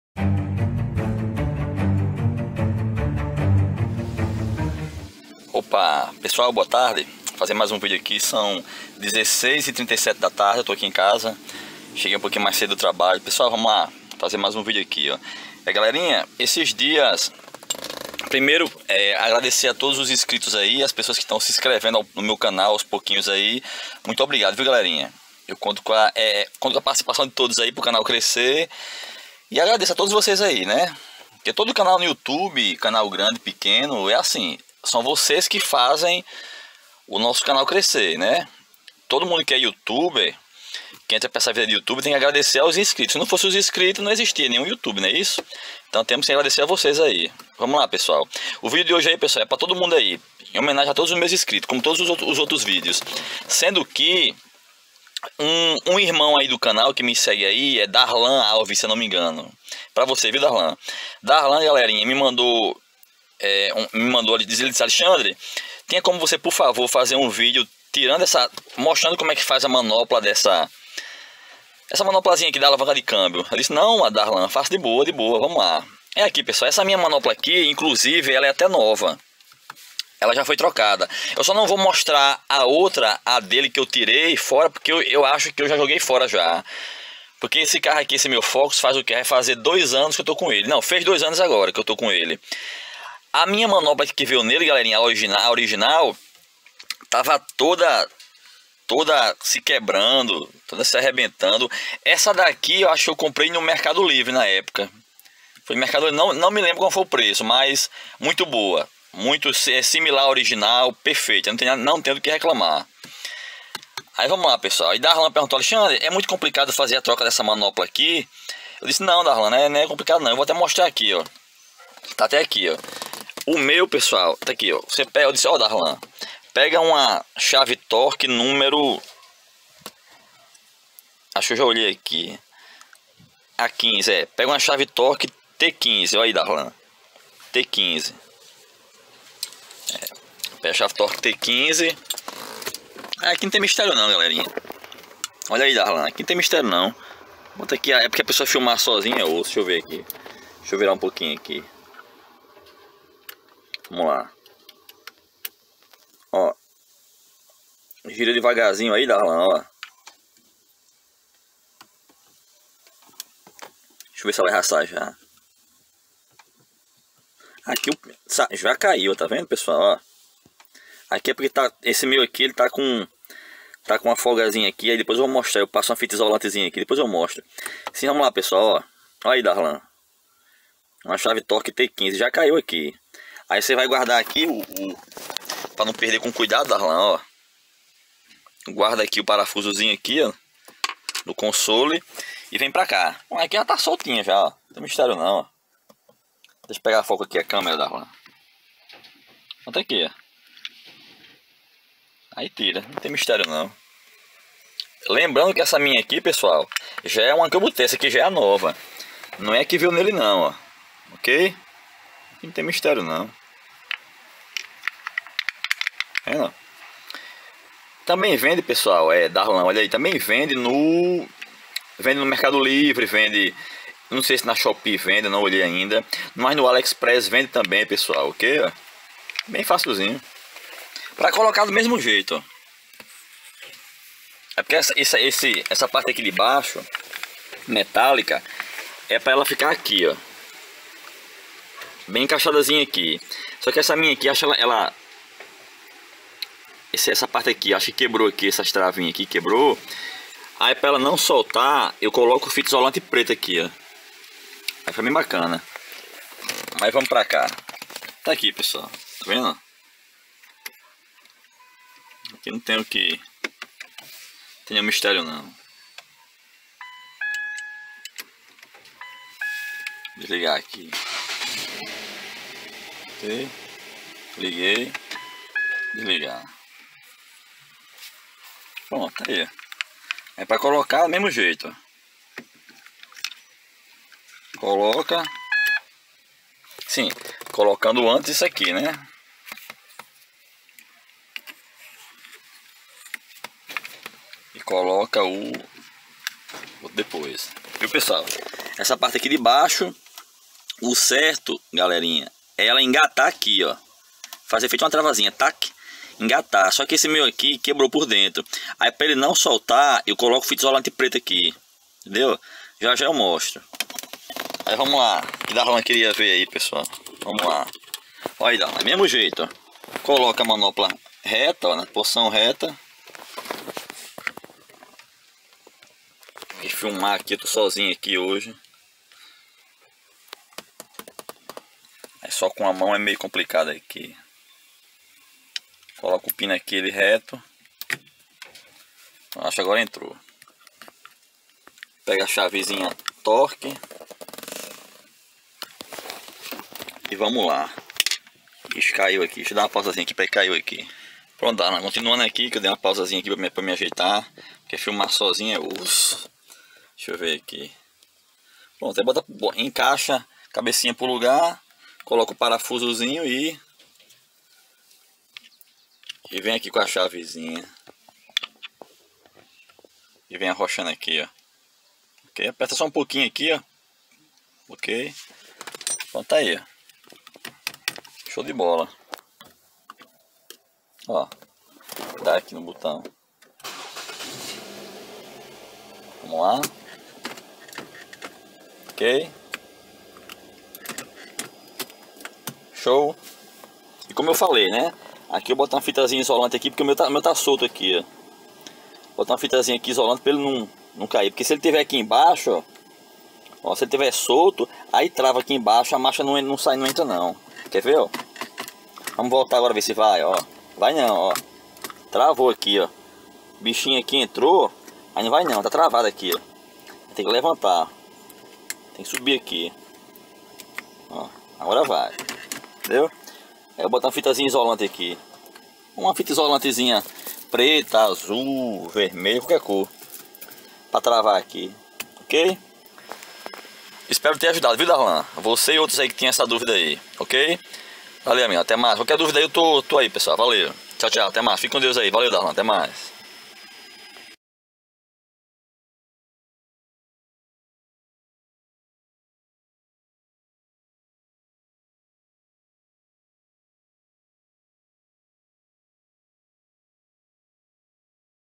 MANOPLA DO CÂMBIO DO FORD FOCUS.
OPAAAAAAA... GALERINHA...FIZ ESSE VÍDEO A PEDIDO DE UM INSCRITO DO CANAL. O MESMO ME FALOU QUE TINHA UMA CERTA DÚVIDA PARA FAZER A SUBSTITUIÇÃO DESSA MANOPLA. ESPERO QUE GOSTEM. ABRAÇO A TODOS E FIQUEM COM DEUS! ATÉ O PRÓXIMO VÍDEO!